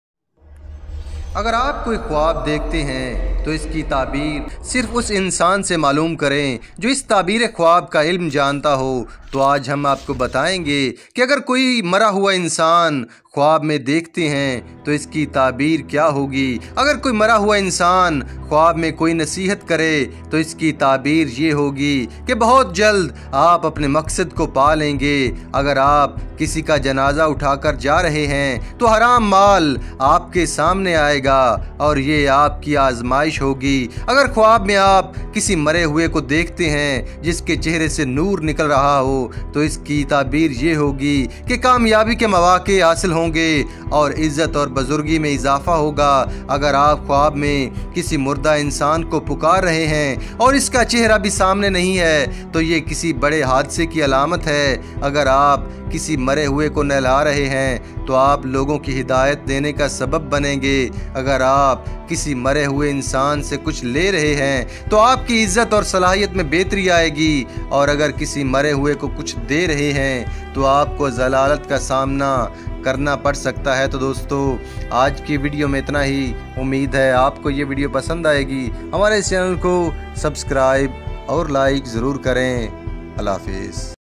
Khwab ki tabir | Khwab Mein Mara Hua Insan dekhne ki tabir | ye khwab dekhne Wala mar jata hai
Channel par apko welcome krty hain
Shukriya
Blagh tv viewers!

Snack video account:
blaghtv